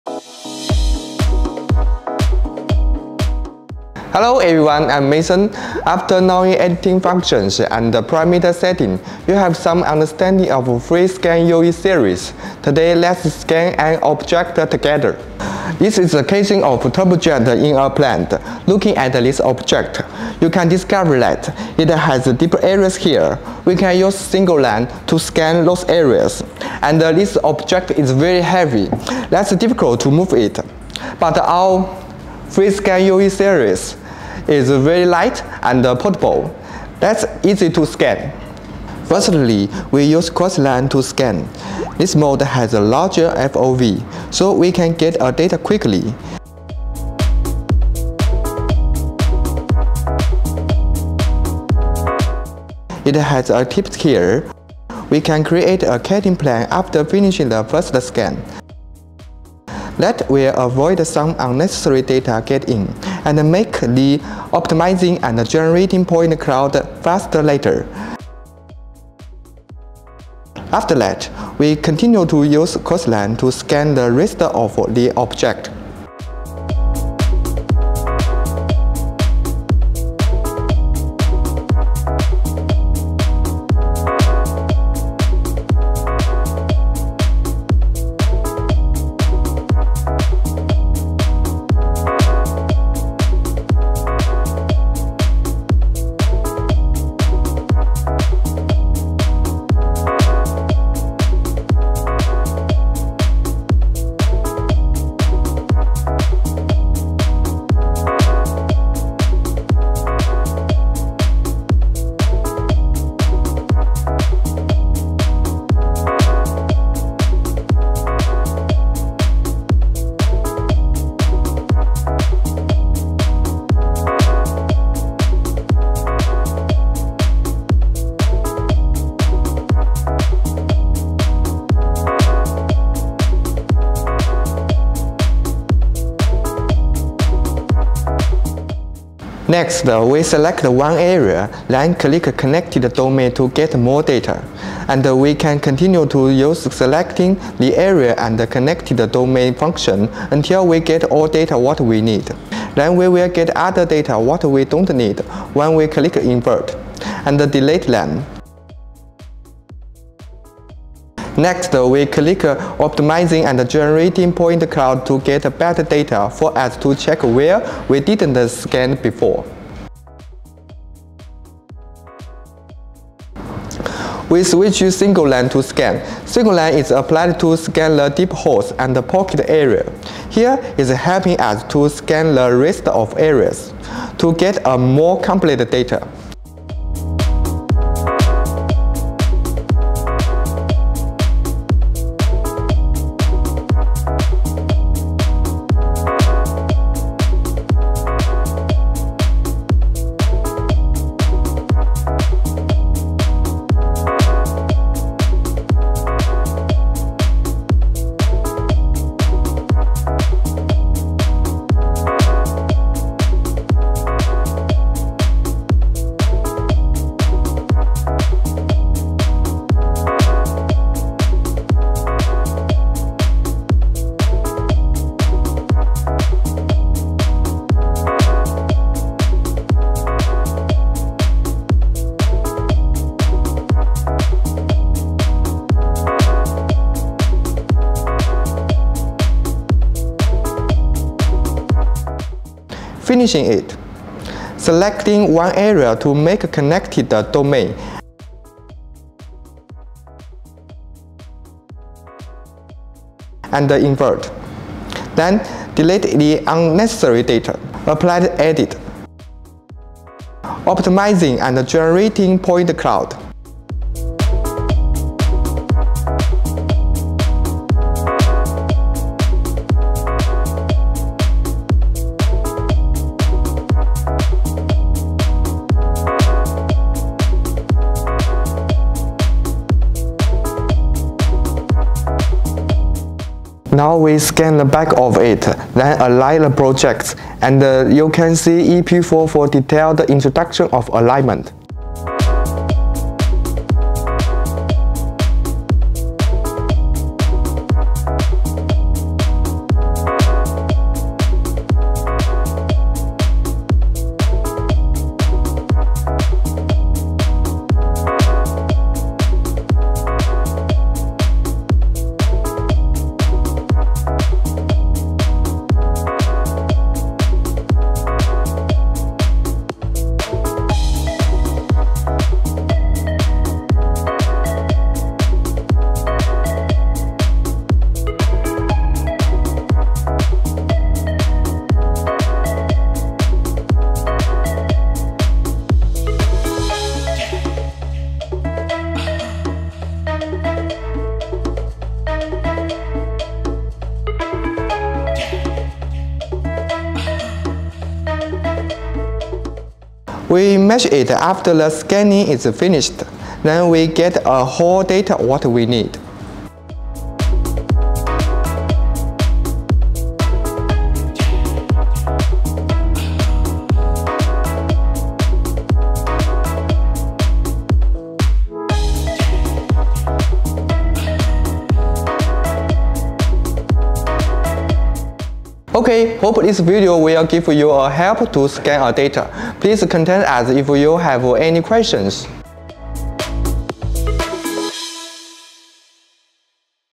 Metrology Solution Salon ep8: How to Scan A Turbojet Engine with FreeScan 3D Scanner
After knowing editing functions and parameter setting, you must have some understanding of FreeScan UE series. In this episode Mason will show you how to scan a turbojet engine.

0:00 The Main Features of FreeScan UE Pro
0:38 Quick Access to Global 3D Data by Multiple Lines Scan Mode
2:53 Data Acquisition of Deep Areas by Single Line Scan Mode
4:43 Build Project 2, Get the 3D Data of the Other Side
5:22 Align Project 1 and 2 to Get the Complete High-Precision 3D Data